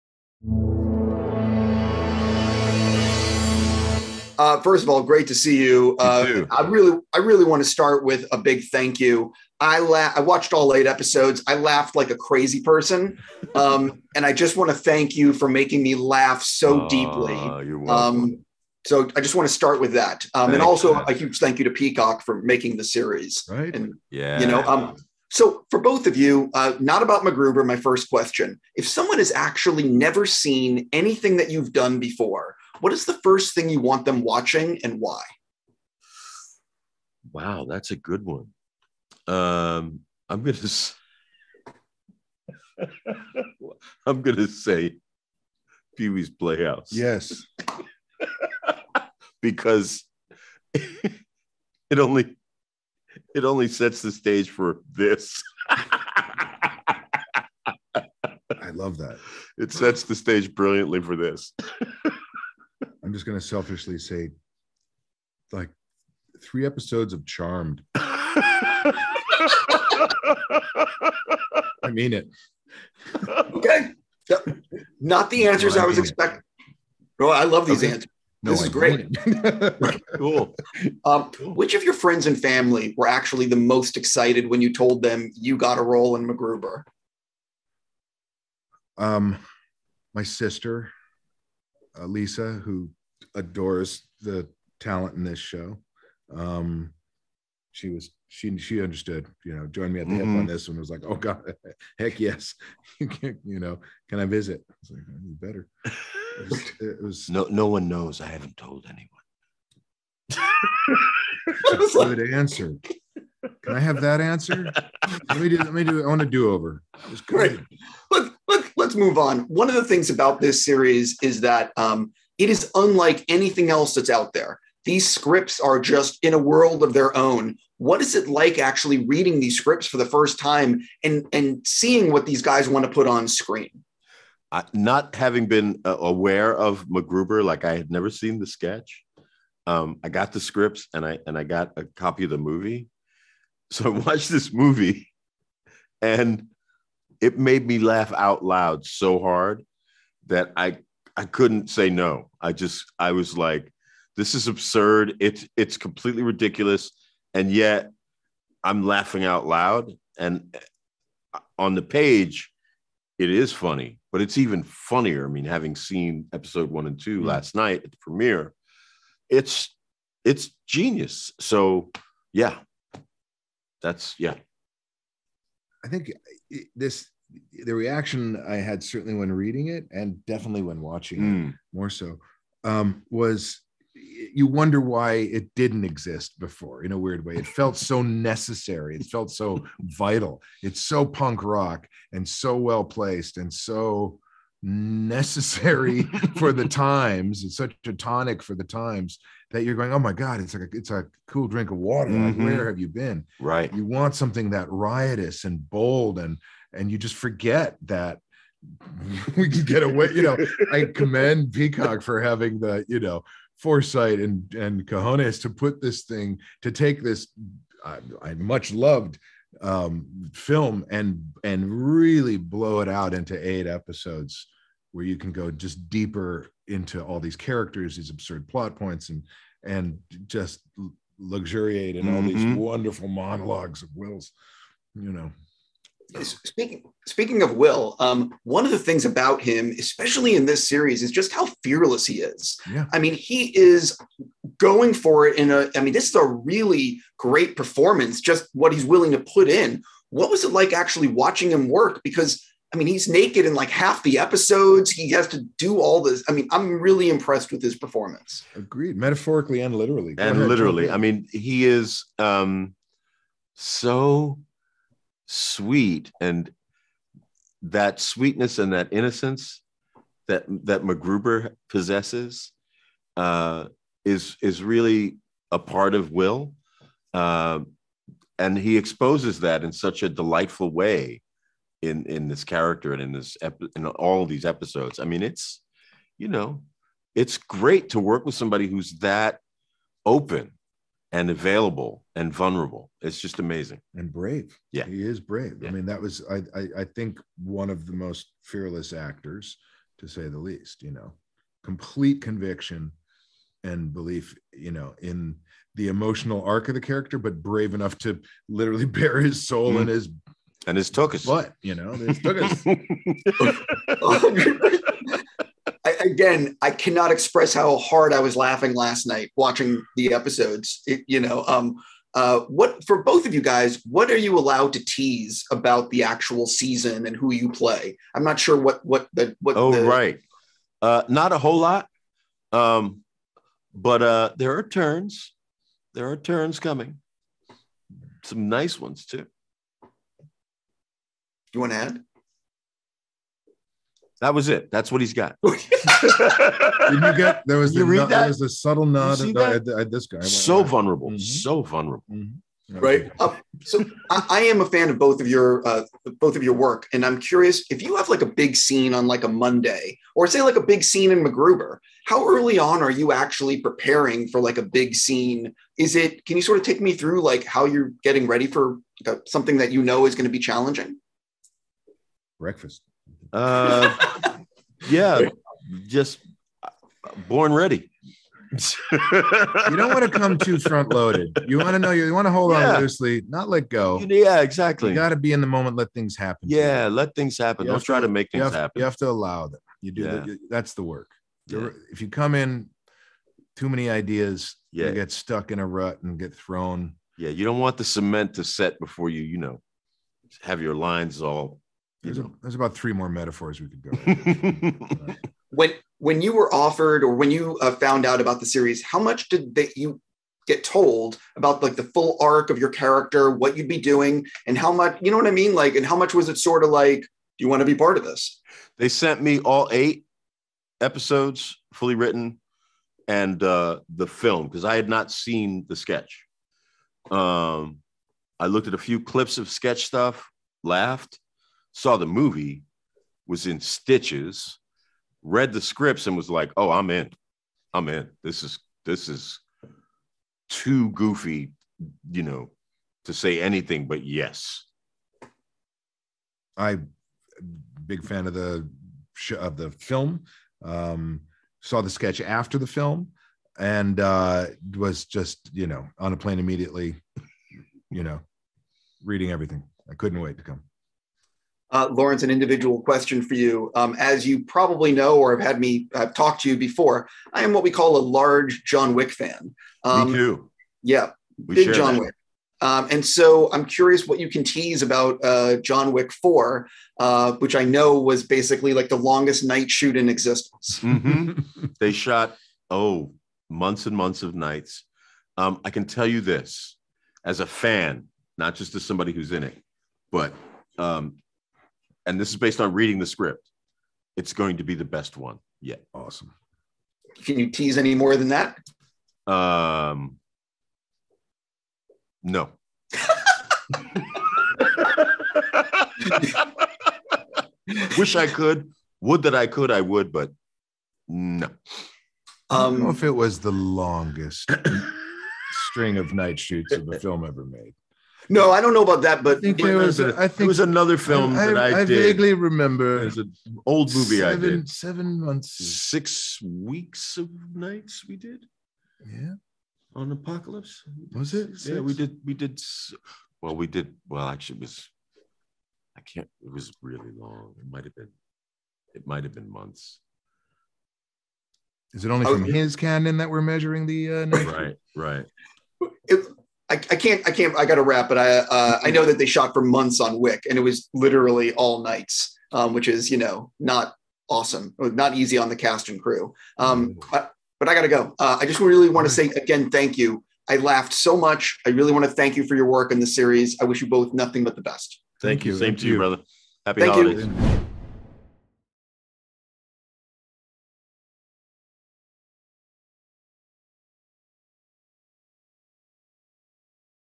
Laurence Fishburne and Billy Zane on MacGruber, Will Forte, and John Wick 4
With MacGruber blessing our timeline with eight new thirty-minute episodes on Peacock, I recently got to speak with Laurence Fishburne (General Barrett Fasoose) and Billy Zane (Brigadier Commander Enos Queeth) about making the series. They talked about why they loved the scripts, what it was like working with Will Forte when he’s so committed to the role, which of their friends and family were the most excited they were part of MacGruber, how much they were told about their characters before signing on, the way they prepare for a big scene, and more. In addition, with Fishburne having recently wrapped on John Wick 4, I asked him what fans can look forward to in the sequel. He said:

    “As a fan, not just as somebody who is in it, and this is based on reading the script, it’s going to be the best one yet.”

Needless to say, this makes me very happy.

If you haven’t seen the MacGruber trailer, the series picks up ten years after the events of the film, where MacGruber (Forte) has been locked up in prison for the brutal killing of Dieter von Cunth (Val Kilmer). But when a mysterious villain from his past, Brigadier Commander Enos Queeth (Billy Zane), threatens the world, MacGruber must reassemble his old team, Vicki St. Elmo (Kristen Wiig) and Dixon Piper (Ryan Phillippe), in order to defeat the forces of evil. As you can easily guess, nothing goes according to plan and it’s fantastic to watch. For more you can read Ross’ review.
MacGruber also stars Sam Elliott (Perry), Joseph Lee Anderson, and Timothy V. Murphy (Constantine Bach). Jorma Taccone, John Solomon, and Forte serve as writers, executive producers, and showrunners of MacGruber. Solomon and Taccone direct all episodes. Lorne Michaels, John Goldwyn, Andrew Singer, and Erin David are also executive producers.

Finally, as a longtime fan of MacGruber, I’m happy to say the new episodes are fantastic and loaded with scenes that will make you laugh out loud. I can’t recommend the series enough. Watch what Laurence Fishburne and Billy Zane had to say in the player above and below is exactly what we talked about.

Laurence Fishburne and Billy Zane
    If someone has never seen anything they’ve done before what is the first thing they should watch and why?
    Which of their friends and family were the most excited they were part of MacGruber?
    How the show is unlike anything else on TV, so what was it like reading the scripts for the first time?
    What is it like working with Will Forte when he is so committed to the role?
    What can they say about their roles?
    How do they like to prepare for a big scene that they know will be challenging?
    How much were they told about their characters arcs before singing on?
    What can Fishburne tease about John Wick 4?